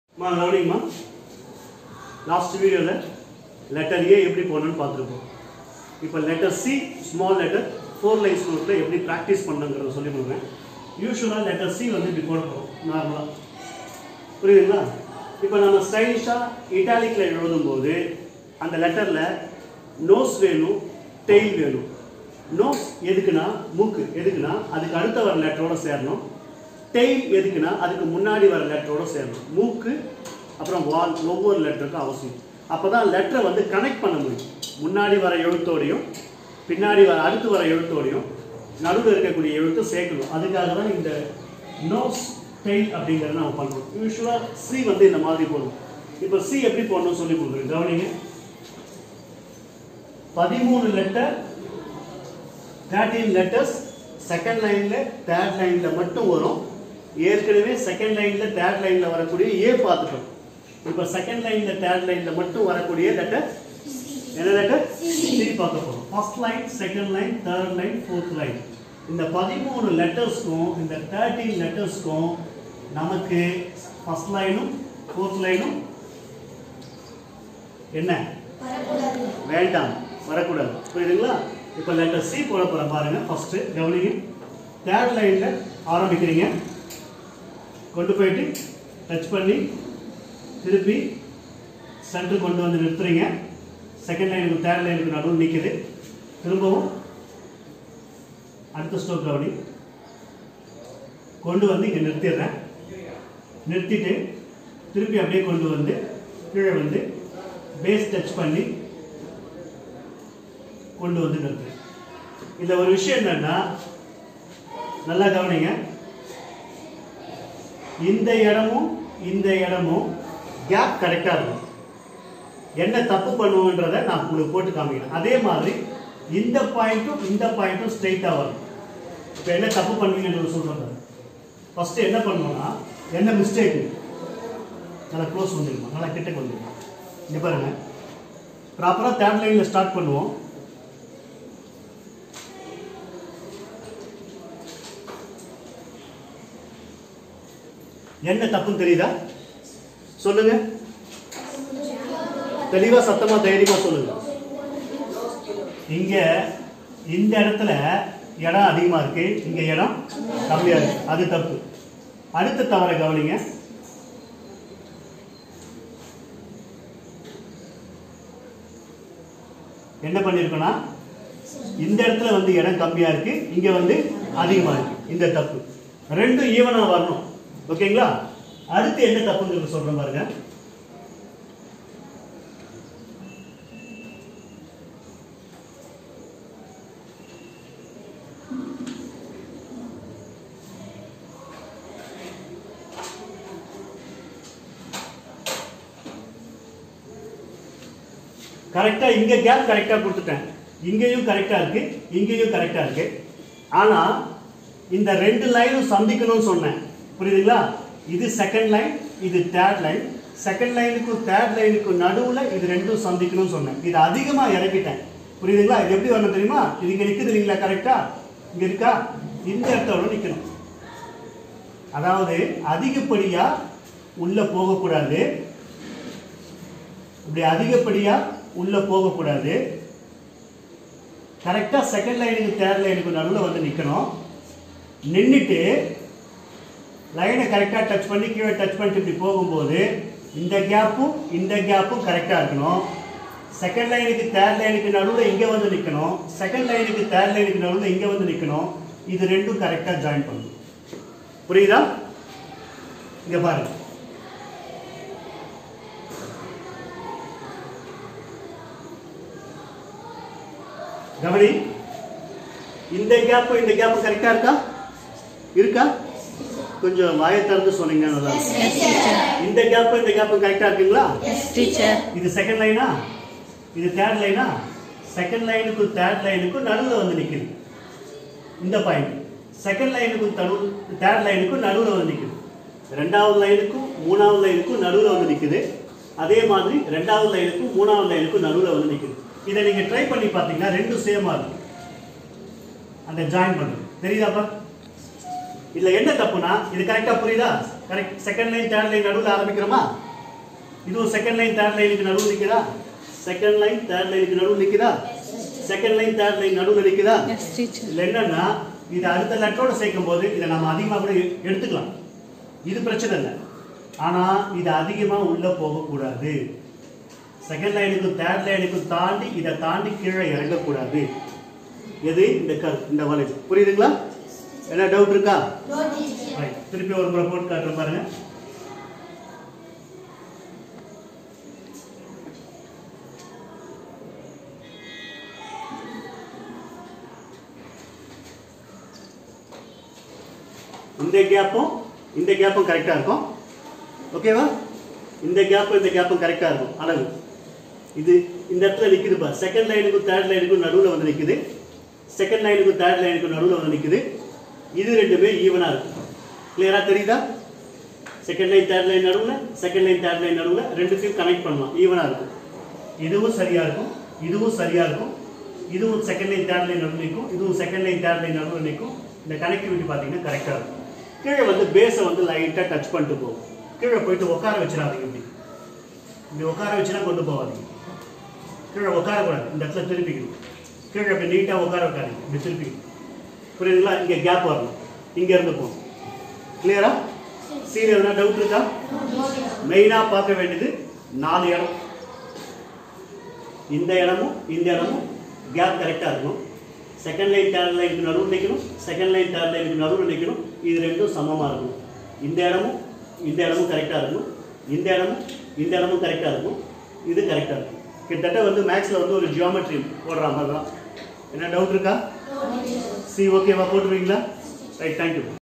21.10.24. ENGLISH HAND WRITING PRACTICE. HOW TO WRITE SMALL LETTER "c". in 4 lines note.
Step by step Practice.